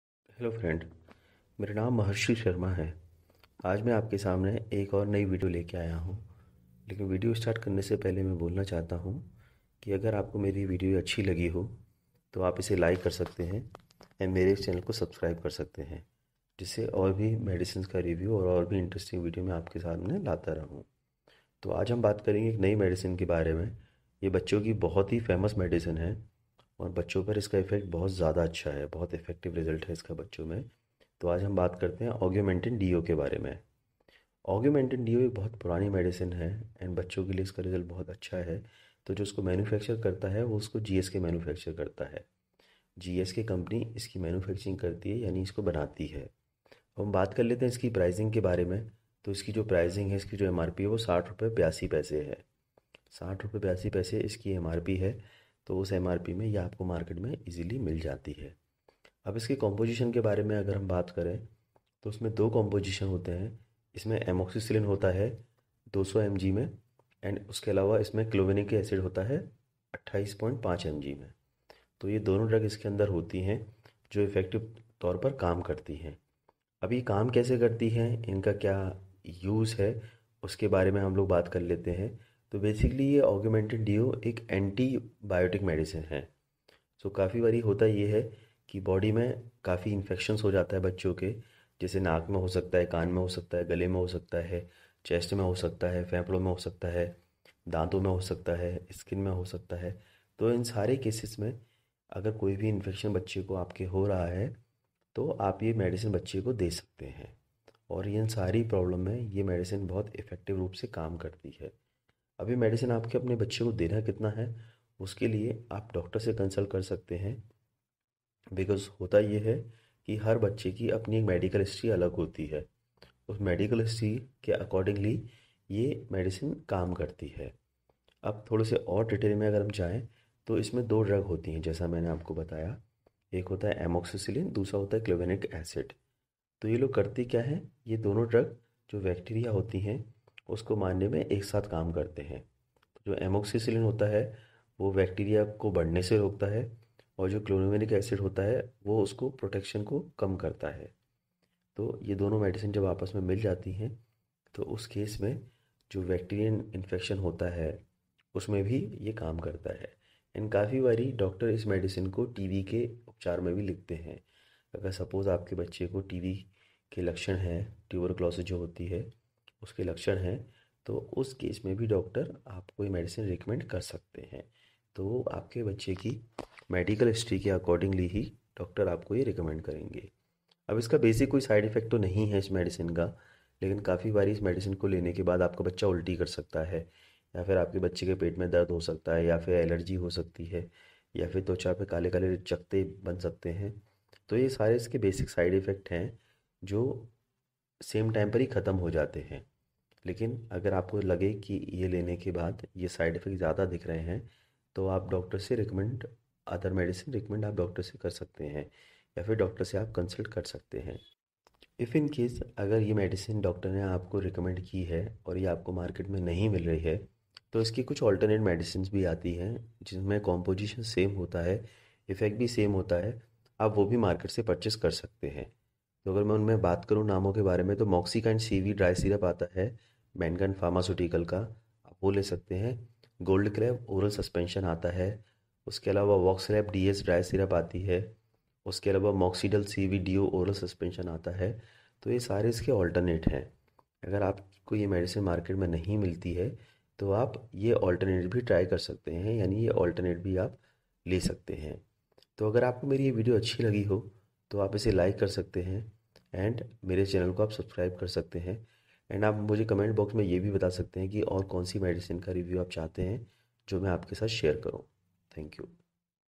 Augmentin DUO | Amoxycillin & Potassium Clavulanate Suspension | augmentin duo syrup hind
Please find detailed review of Augmentin DUO suspension. This is one of the best medicine for your children for any type of becterial infection. Before taking any medicine please consult with your doctor & health care advisor.

augmentin duo syrup side effects
augmentin duo dry syrup
augmentin duo syrup hindi
augmentin duo syrup doses
augmentin duo syrup for paediatric